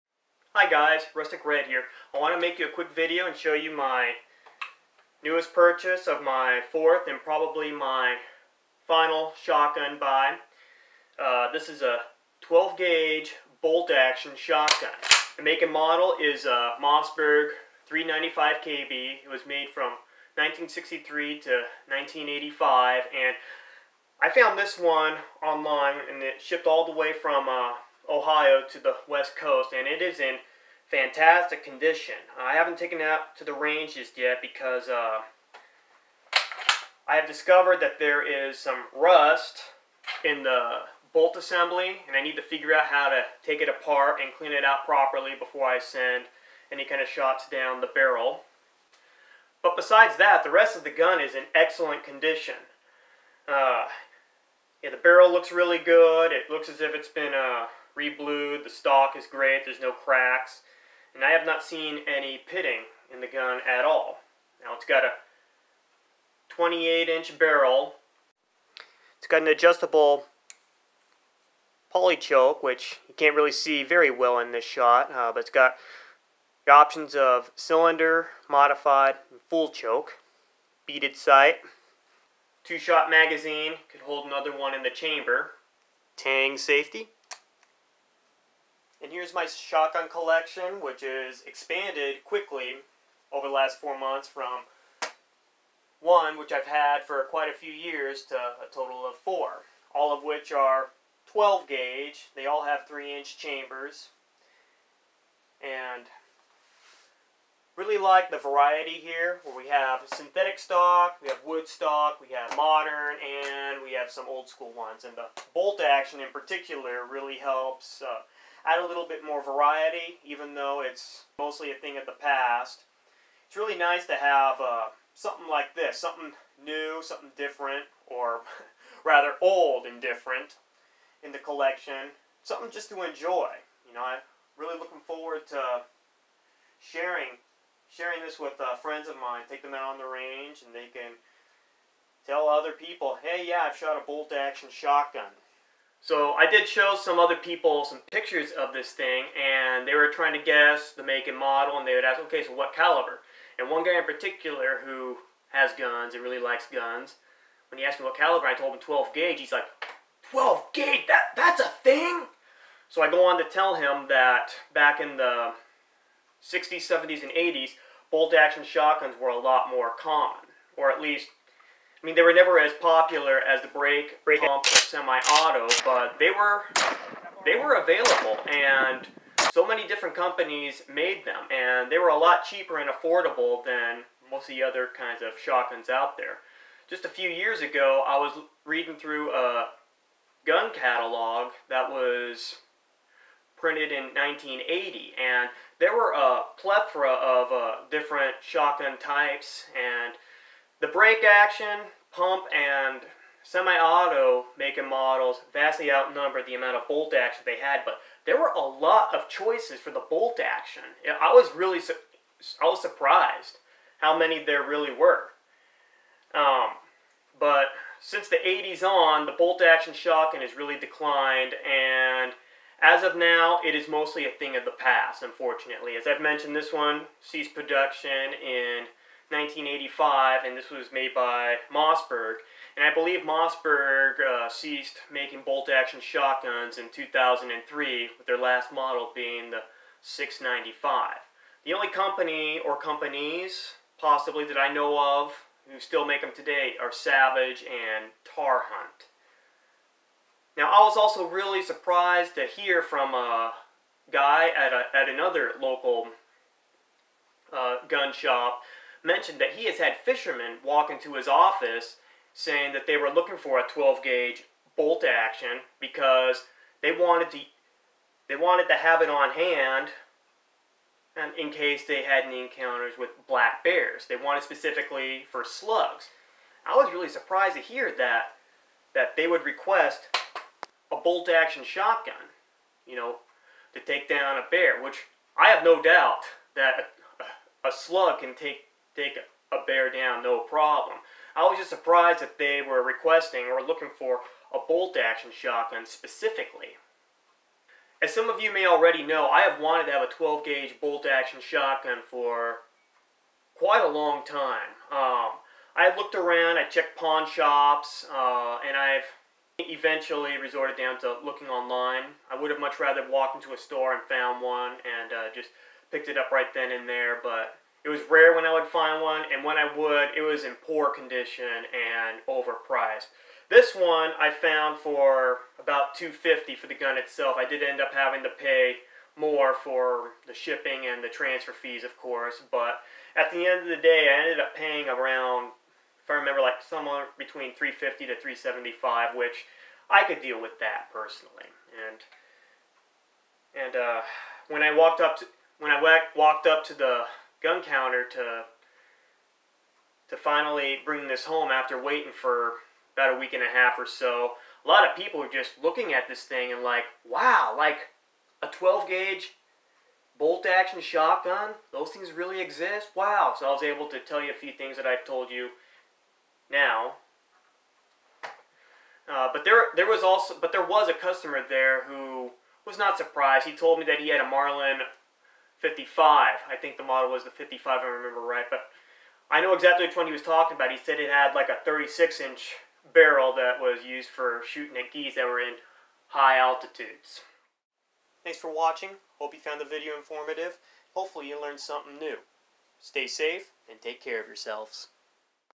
Bolt Action Shotgun: Mossberg 395 KB.
I've wanted a 12 gauge bolt action for a long time and found one online for a good price.